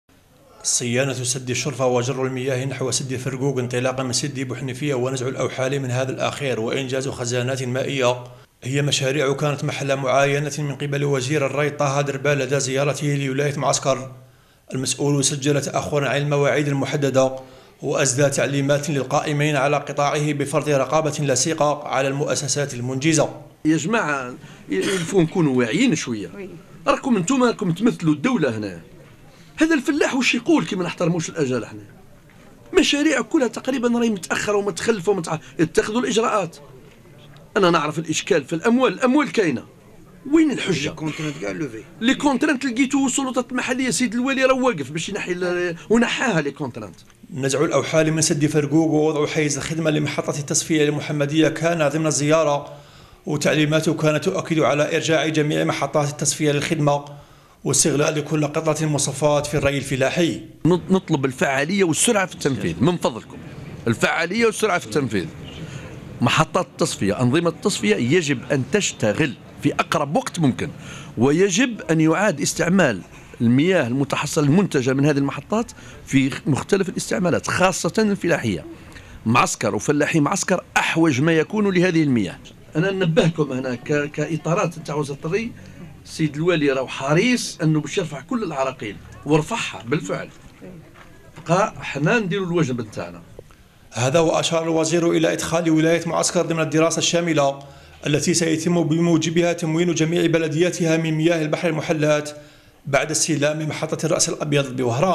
معسكر ...وزير الري يأمر برقابة صارمة في متابعة إنجاز المشاريع
HD 10992/5000
SD 10921/27500/V